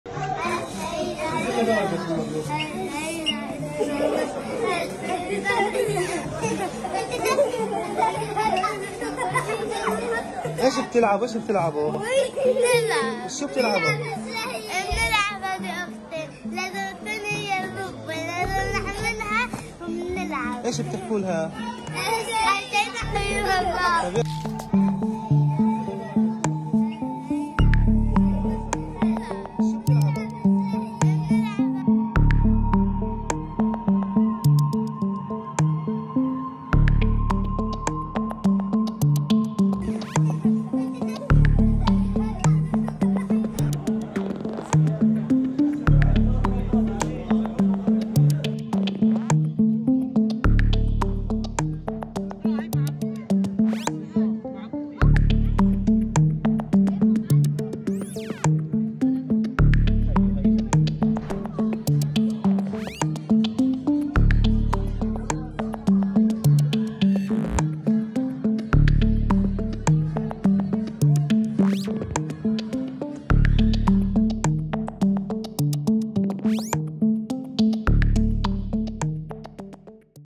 أطفال غزة يُحوّلون معاناتهم إلى مصدر للعب
من واقع الحرب.. أطفال غزة يبتكرون من مأساتهم ومعاناتهم ألعاباً لقضاء وقتهم في أحد المستشفيات، الذي تحوّل  إلى مركز لجوء